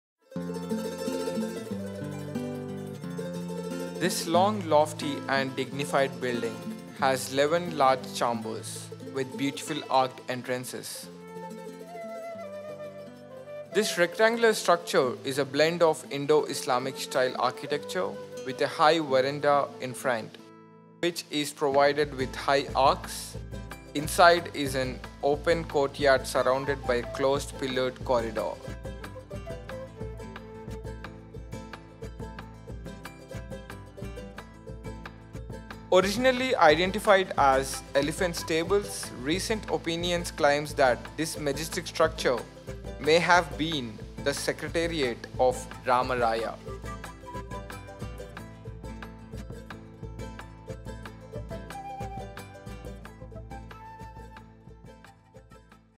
Royal Elephant Stables | Königlichen Elefantenställe | Hampi
The Elephant Stable in Hampi is an impressive structure that was used to provide shelter for the royal elephants of the Vijayanagara Empire. The elephant stable is located in the area that lies just outside the Zenana Enclosure.